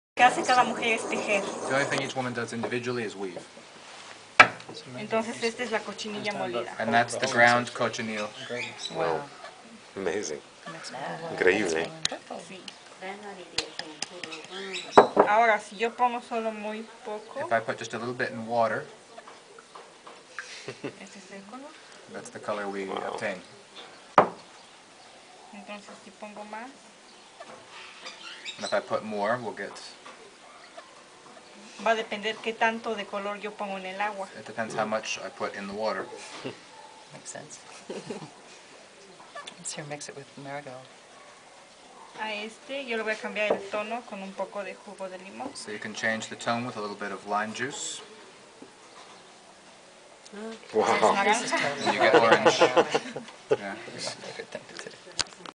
Grana Cochinilla
They still use traditional dying techniques in Teotitlan del Valle. This weaver showed us different ways to use the grana cochinilla (a bug that grows on cactus) along with other ingredients to make different tones of red. Amazing!